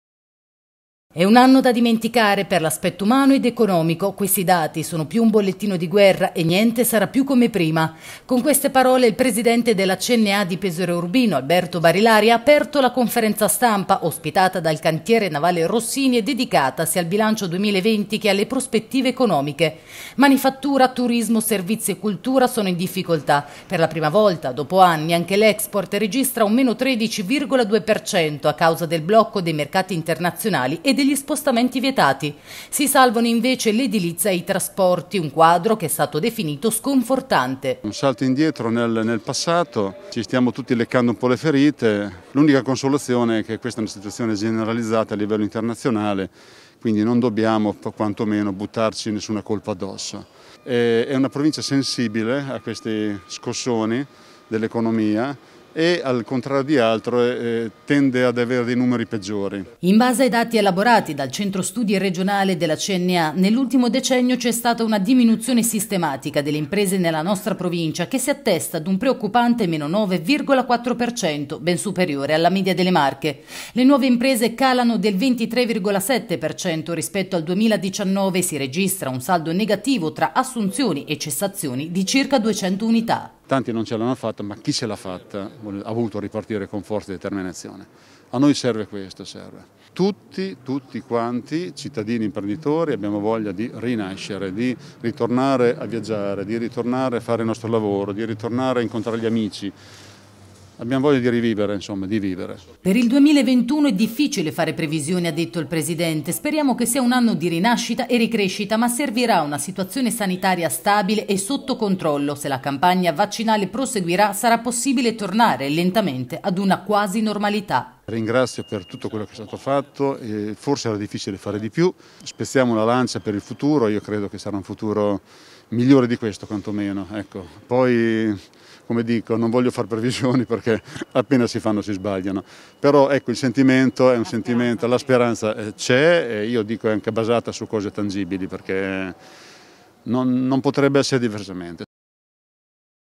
Bilancio Cna: "Un anno da dimenticare"
Da modello virtuoso a provincia in affanno. E’ un quadro preoccupante quello delineato ieri mattina dalla CNA di Pesaro e Urbino che ha fatto un bilancio dell’anno appena passato. Si salvano soltanto i settori dell’edilizia e dei trasporti.